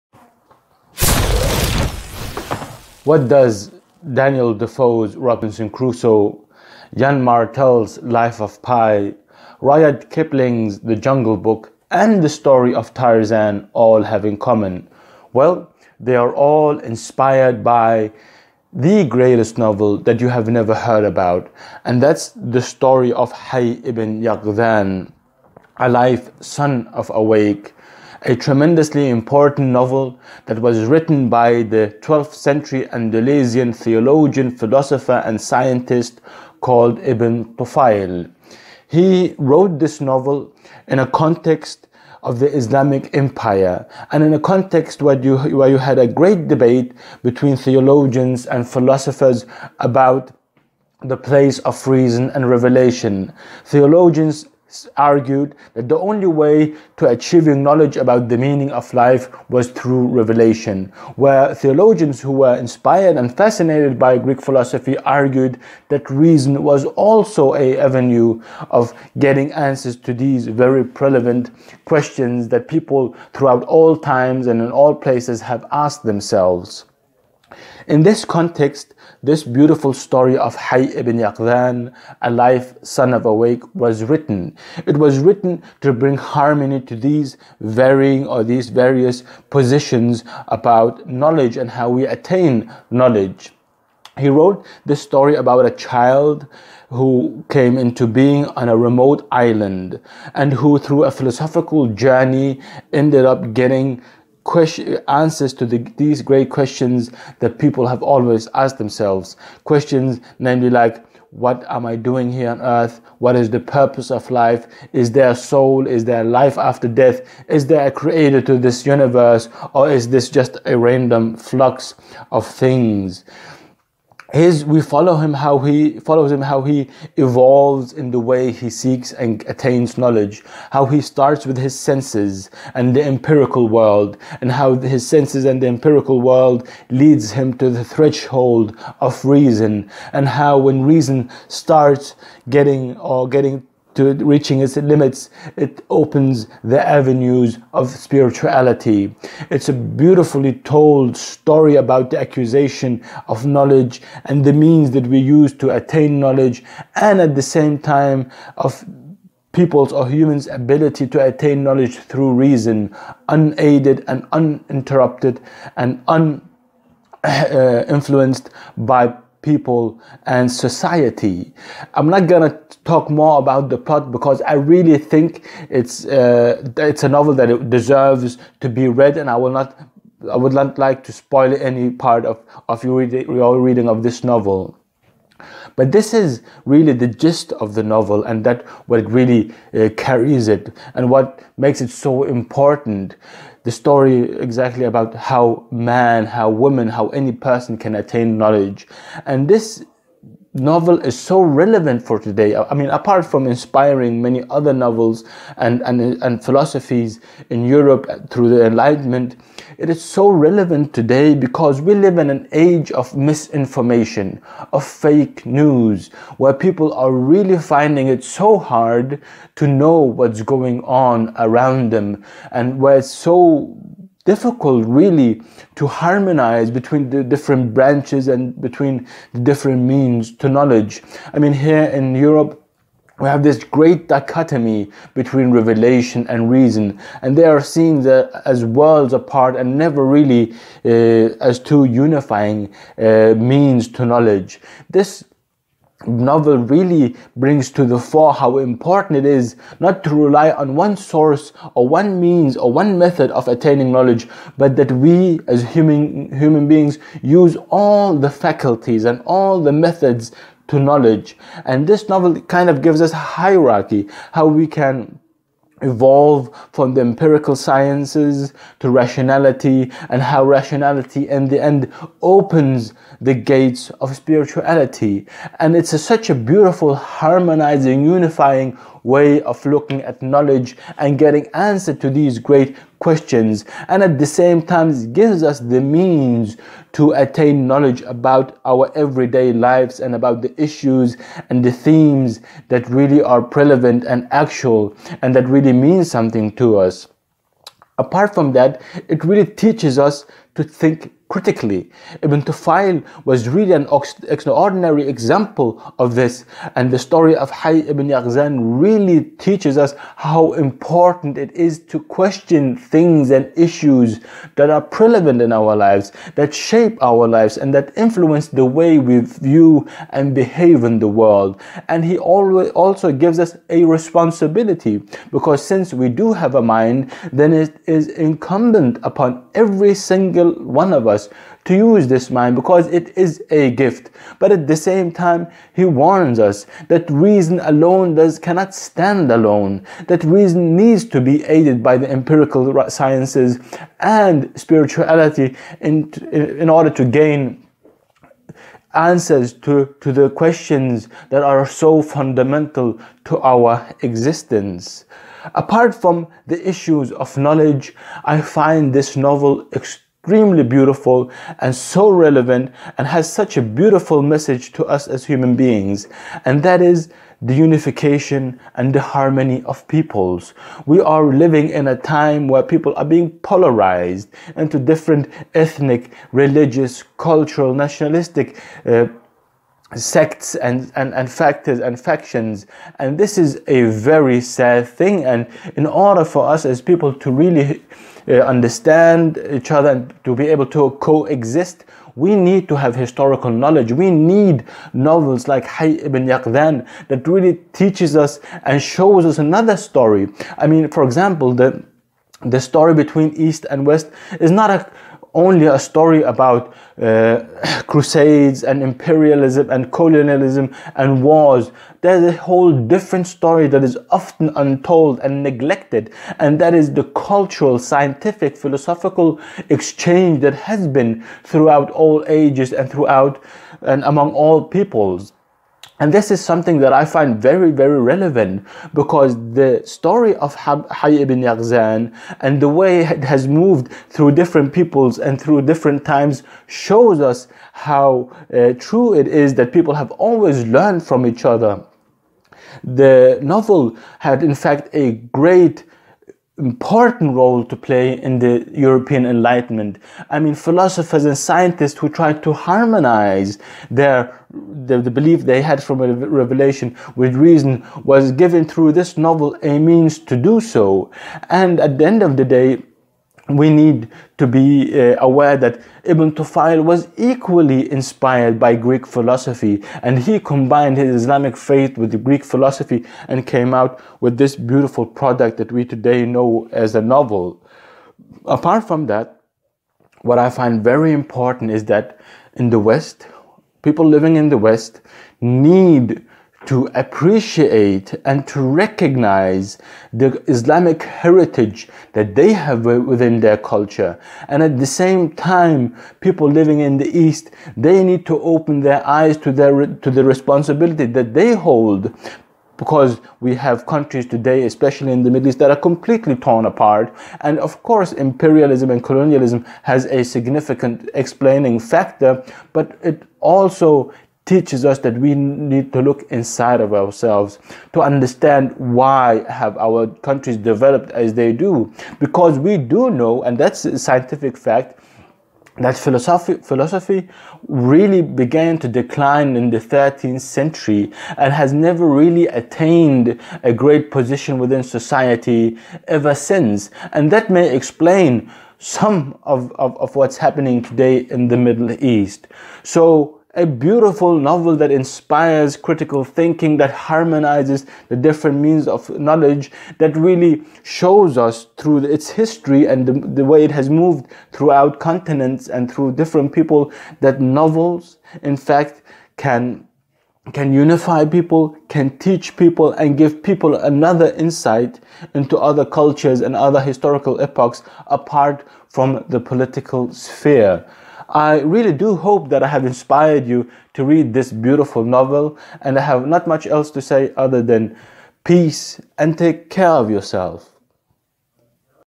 Literature for Life: Ibn Tufayl 'Alive Son of Awake'
A review of  Ibn Tufayl's great philosophical novel 'Alive Son of Awake'